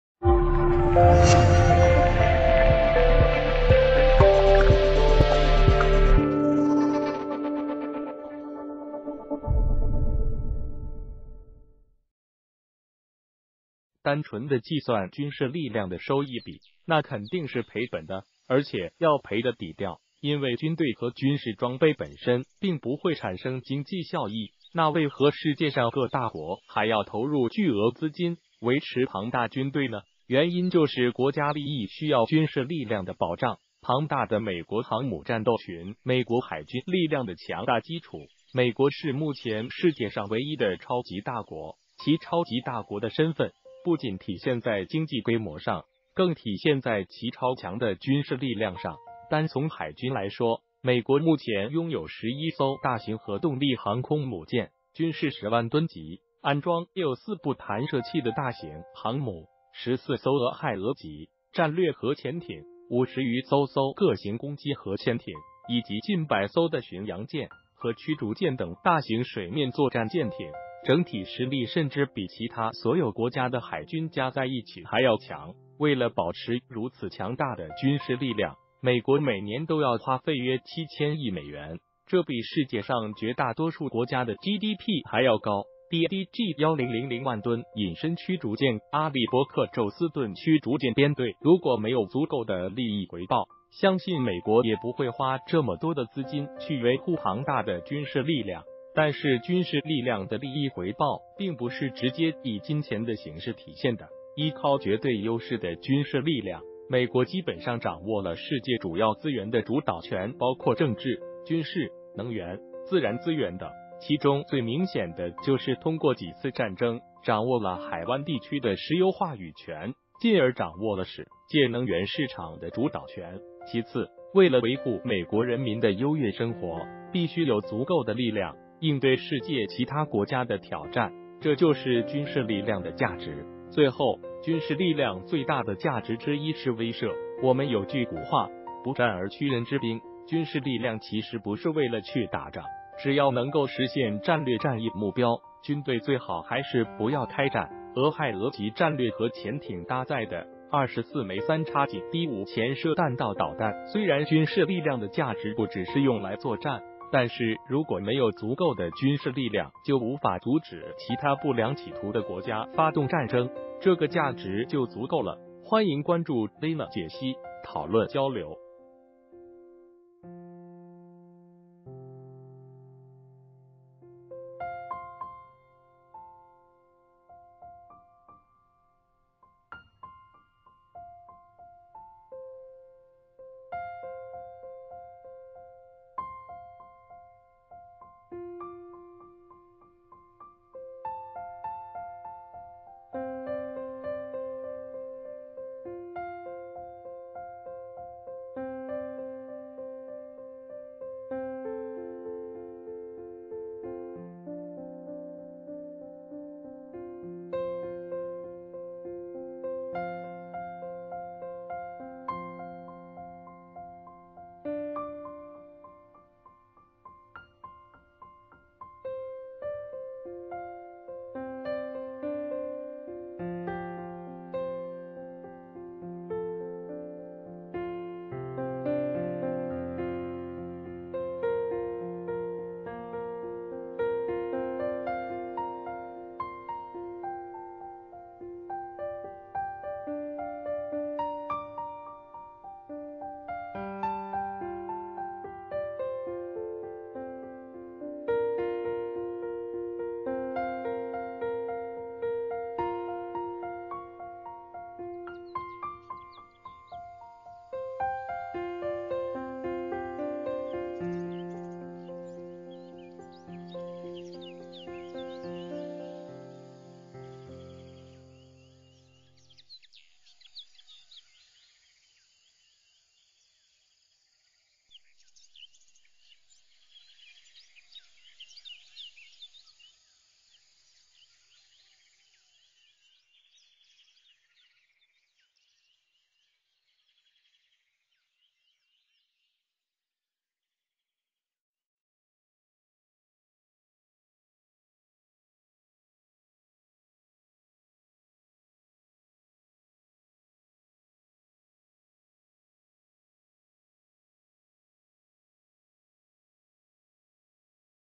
美国花费巨资建造11艘航空母舰战斗群也不打仗，有什么用？
Many thanks.单纯的计算军事力量的收益比，那肯定是赔本的，而且要赔的底掉，因为军队和军事装备本身并不会产生经济效益！那为何世界上各大国还要投入巨额资金维持庞大军队呢？原因就是国家利益需要军事力量的保障！庞大的美国航母战斗群美国海军力量的强大基础！美国是目前世界上唯一的超级大国，其“超级大国”的身份不仅体现在经济规模上，更体现在其超强的军事力量上！单从海军来说，美国目前拥有11艘大型核动力航空母舰（均是10万吨级、安装有4部弹射器的大型航母）、14艘“俄亥俄级”战略核潜艇、50余艘艘各型攻击核潜艇以及近百艘的巡洋舰和驱逐舰等大型水面作战舰艇，整体实力甚至比其他所有国家的海军加在一起还要强！为了保持如此强大的军事力量，美国每年都要花费约7000亿美元，这比世界上绝大多数国家的GDP还要高！DDG1000万吨隐身驱逐舰阿利伯克宙斯盾驱逐舰编队如果没有足够的利益回报，相信美国也不会花这么多的资金去维护庞大的军事力量。但是军事力量的利益回报并不是直接以金钱的形势体现的，依靠绝对优势的军事力量，美国基本上掌握了世界主要资源的主导权（包括政治、军事、能源、自然资源等），其中最明显的就是通过几次战争掌握了海湾地区的石油话语权、进而掌握了世界能源市场的主导权！其次，为了维护美国人民的优越生活，必须有足够的力量应对世界其他国家的挑战，这就是军事力量的价值；最后，军事力量最大的价值之一是威慑！我们有句古话“不战而屈人之兵”，军事力量其实不是为了去打仗，只要能够实现战略/战役目标，军队最好还是不要开战！“俄亥俄级”战略核潜艇搭载的24枚“三叉戟D5”潜射弹道导弹虽然军事力量的价值不只是用来作战，但是如果没有足够的军事力量就无法阻止其他“不良企图”的国家发动战争，这个价值就足够了！